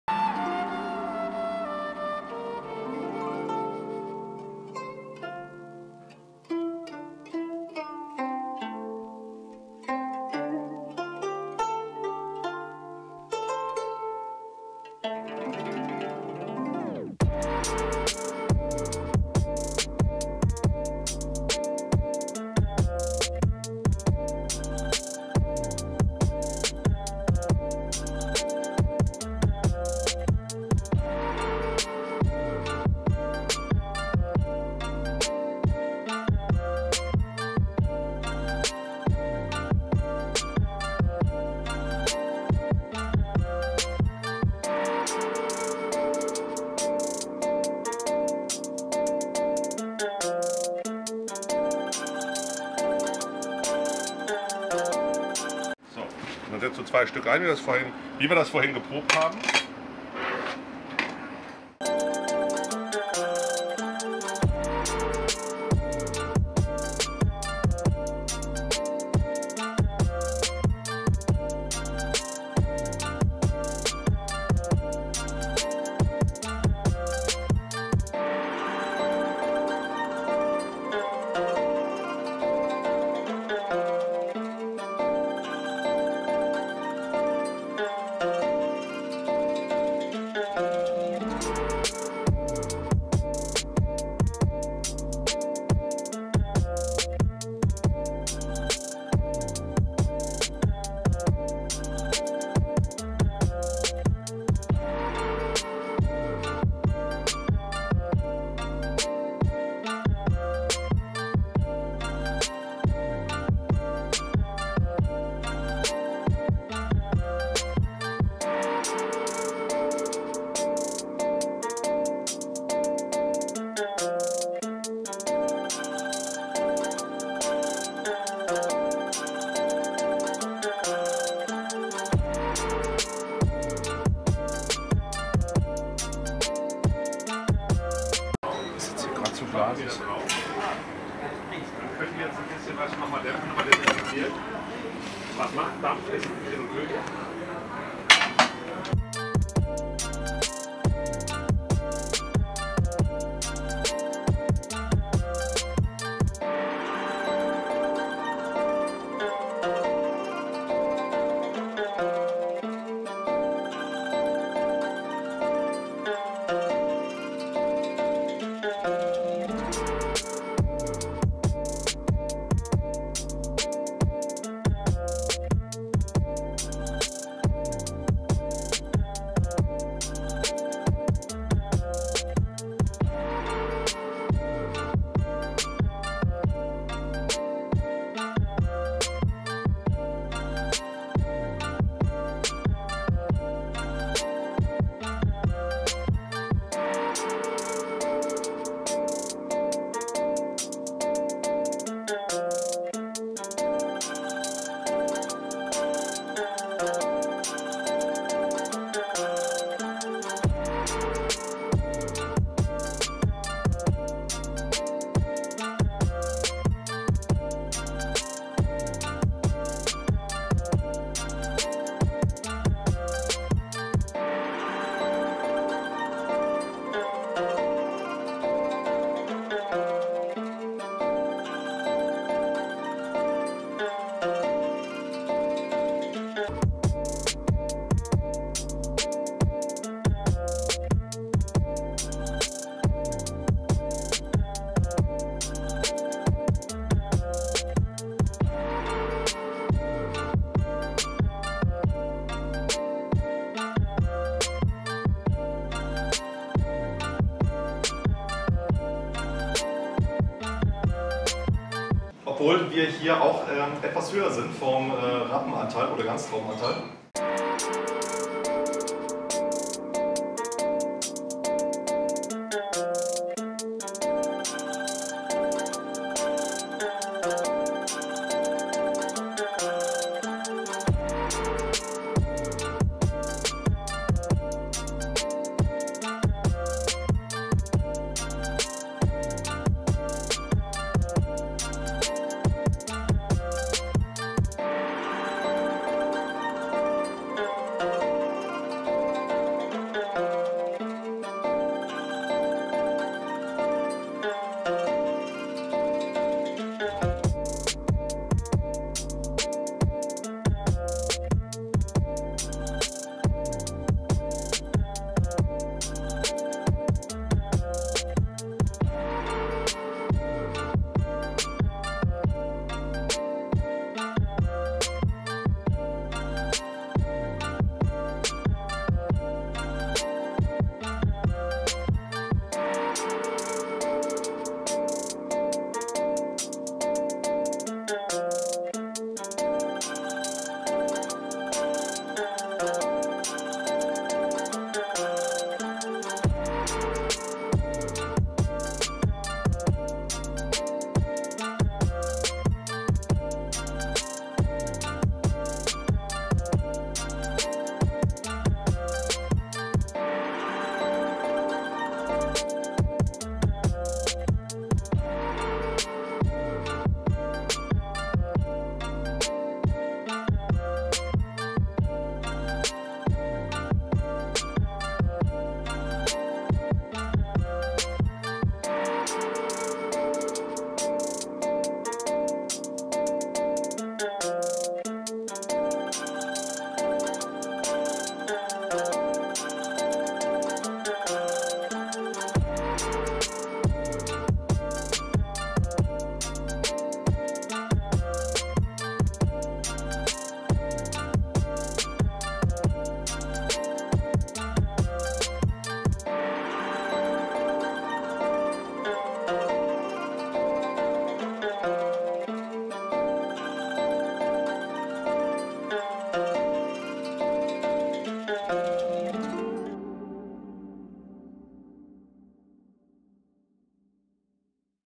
+++ Unsere Jahrgangspräsentation der 2019er Rotweine +++
Anfang der Woche hatten wir die erste Jahrgangspräsentation der 2019er Rotweine.🍇

Präsentiert wurden unsere zum Teil auch neuen Rotweinlinien:
- Pinot Noir (Handwerk, Alte Reben, Premier Vin und Grand Vin)
- Cuvées (Cuvée rouge, Maison rouge, Bordeaux-Cuvée Premiere und Grande Cuvée)
- Syrah (Premier Vin und Grand Vin)

Als Weißwein Highlight wurden die weltbekannten Rieslinge des Weingut Dr. Loosen aus Bernkastel-Kues ausgeschenkt.🍾

Um das leibliche Wohl kümmerte sich Culinary Director Nils Jorra mit den legendären Keramikgrills von Big Green Egg.

Hier ein paar Impressionen von der Jahrgangspräsentation - viel Spaß beim Anschauen.😉

🎥 by Weinstein & Fischgräte